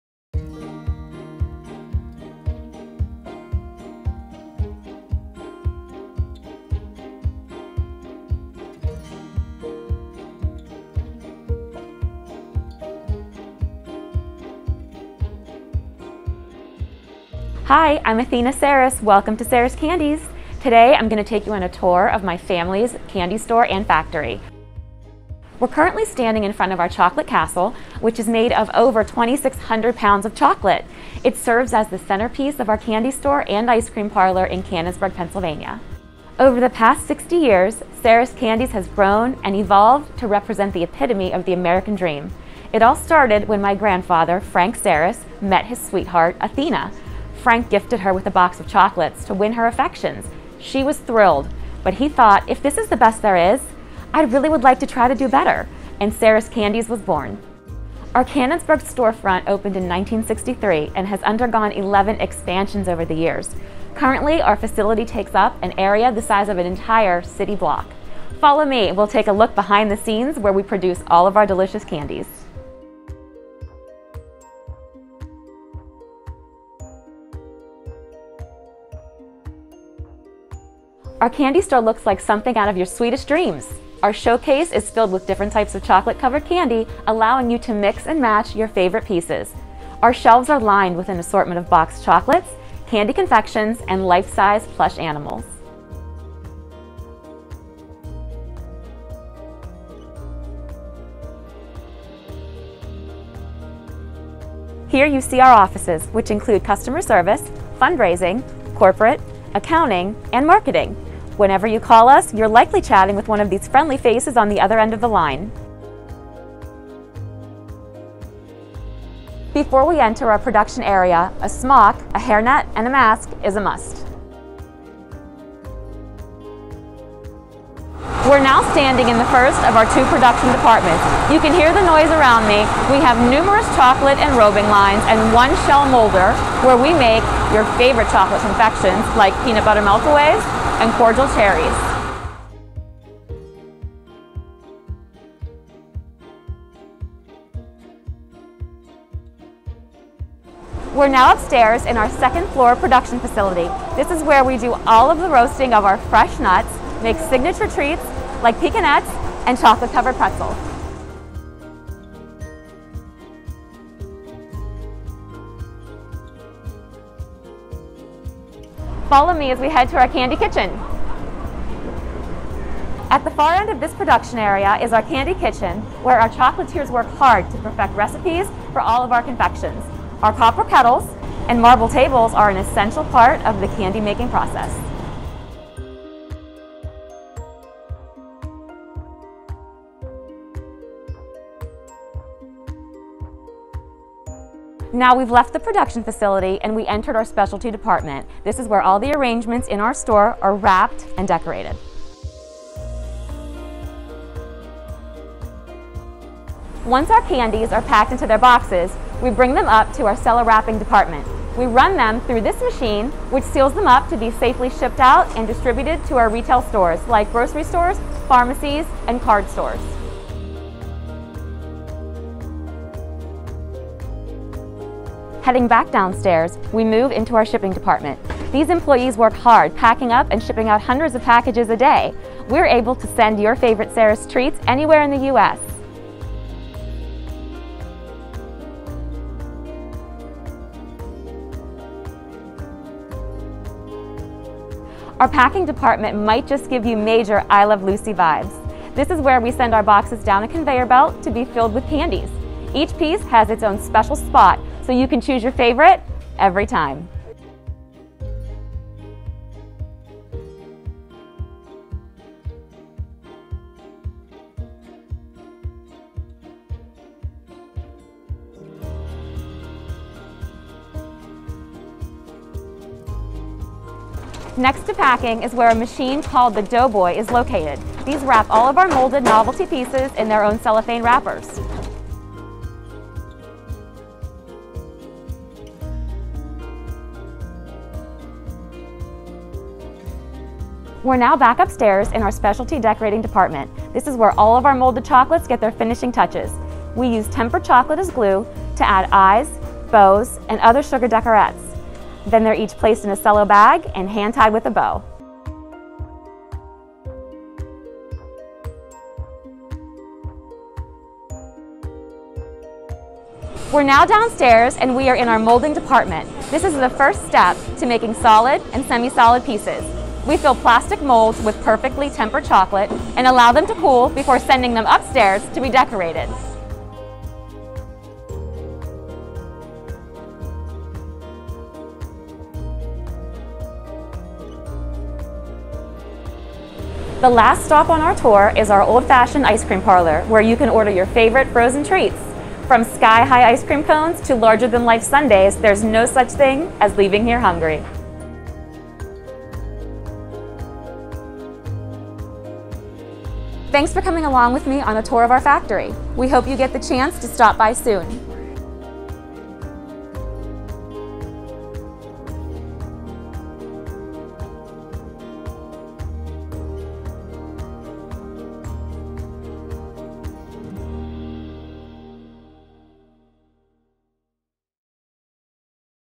Behind the Scenes: Factory Tour
Take a look behind the scenes of our candy store and factory located in Canonsburg, PA.